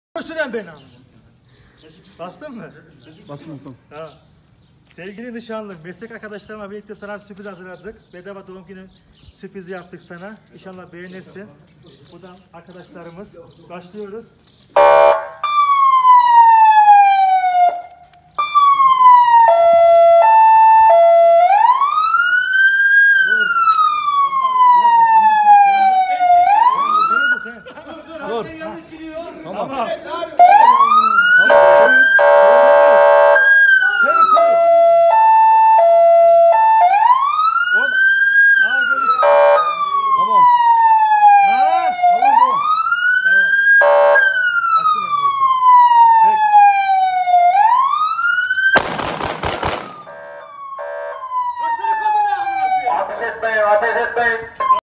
Asker den nişanlısına doğum günü sürprizi.
Askerimizden nişanlısına supriz